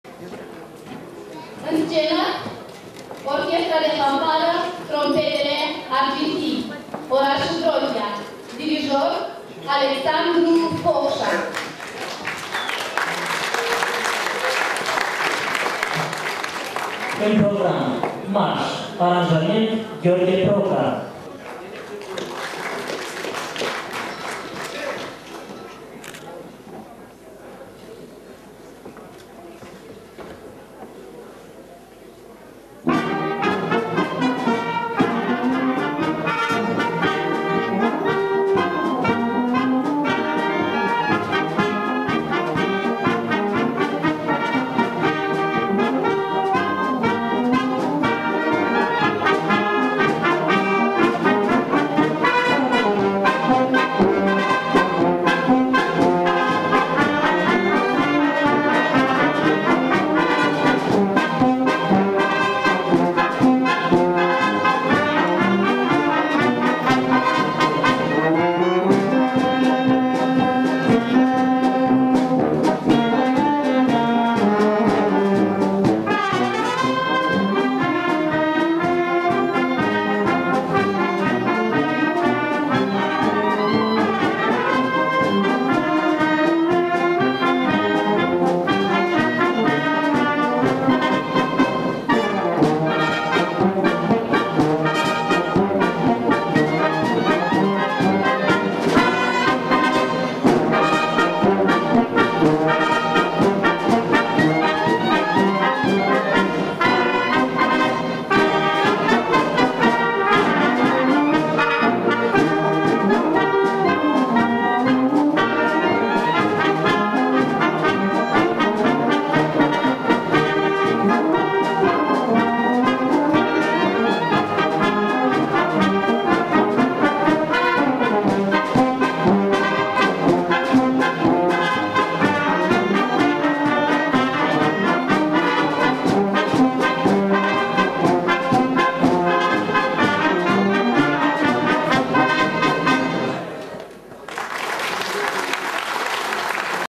Giţă Proca fanfara Marş.mpg
o variantă instrumentală de muzică populară pentru fanfară în varianta de marş